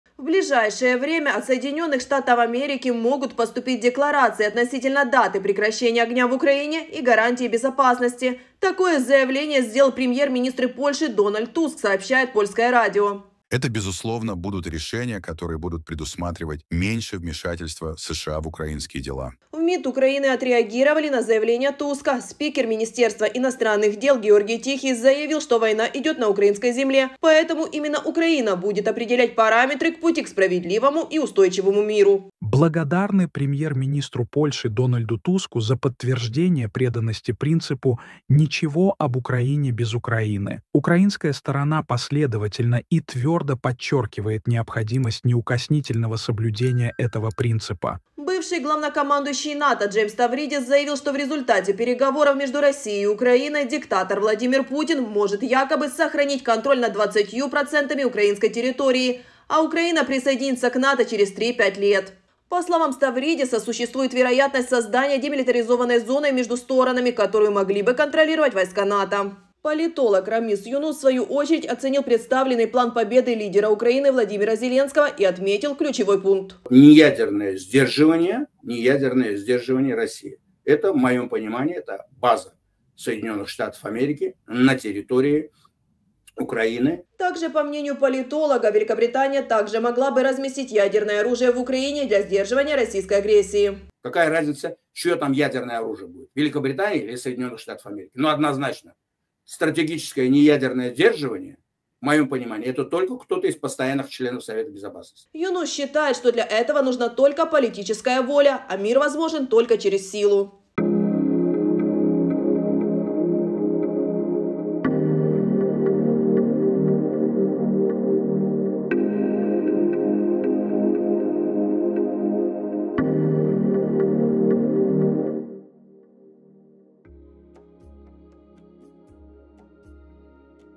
🔥Дата КОНЦА войны! Названы ВАЖНЫЕ условия для ПЕРЕМИРИЯ. Решение Трампа с НАТО
RU - В ближайшее время от США могут поступить декларации о датах прекращения огня в Украине и гарантиях безопасности. Подробности узнавайте в нашем сюжете.

UA - Найближчим часом від США можуть надійти декларації про дати припинення вогню в Україні та гарантії безпеки. Подробиці дізнавайтесь у нашому сюжеті.